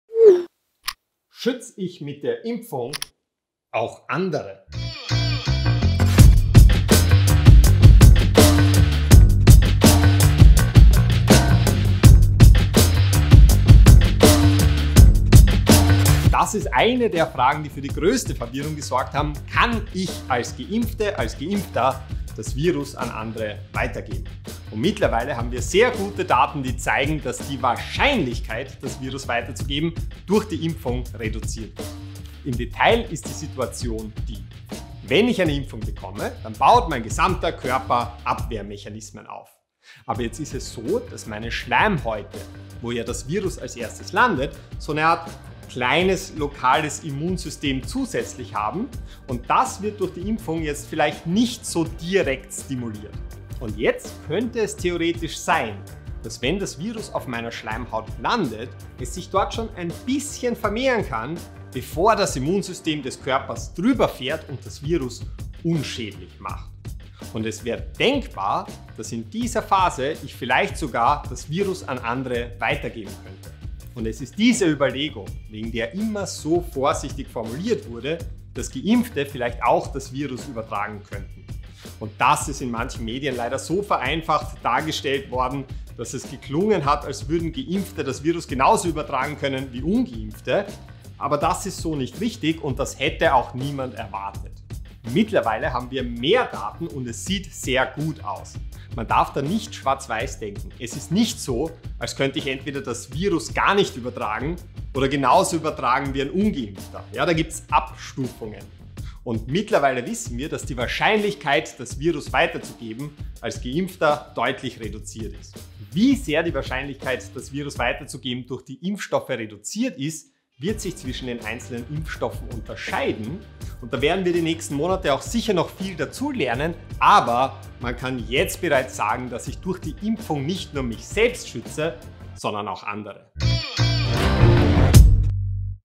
Corona-Faktencheck mit Martin Moder: Schützt meine Impfung auch andere?
Kurz, knapp und knackig: Science Buster und Molekularbiologe Martin Moder klärt im Faktencheck der Österreichischen Akademie der Wissenschaften, ob eine Impfung gegen das Coronavirus auch andere schützt und nicht nur mich.

Single dose of Pfizer BioNTech vaccine reduces asymptomatic infections and potential for SARS-CoV-2 transmission